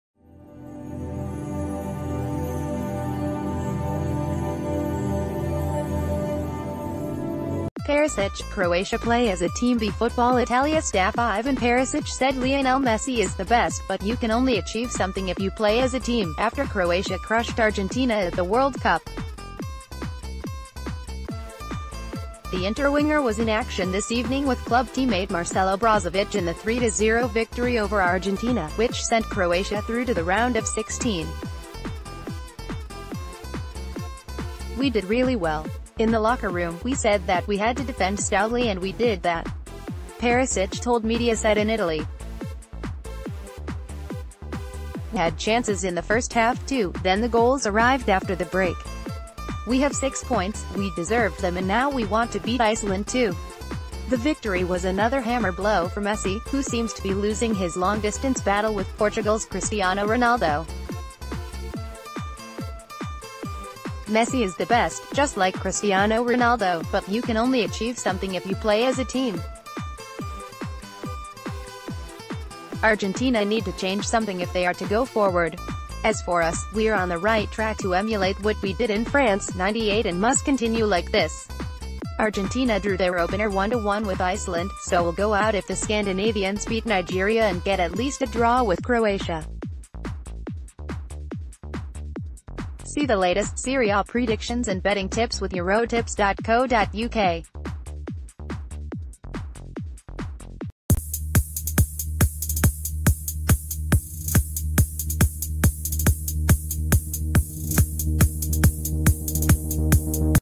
Breaking News | TODAY'S FRONT PAGES
Perisic: 'Croatia play as a team'By Football Italia staff Ivan Perisic said Lionel Messi is “the best, but you can only achieve something if you play as a team” after Croatia crushed Argentina at the World Cup. The Inter winger was in action this evening with club teammate Marcelo Brozovic in the 3-0 victory over Argentina, which sent Croatia through to the Round of 16. “We did really well. In the locker room, we said that we had to defend stoutly and we did that,” Perisic told Mediaset in Italy...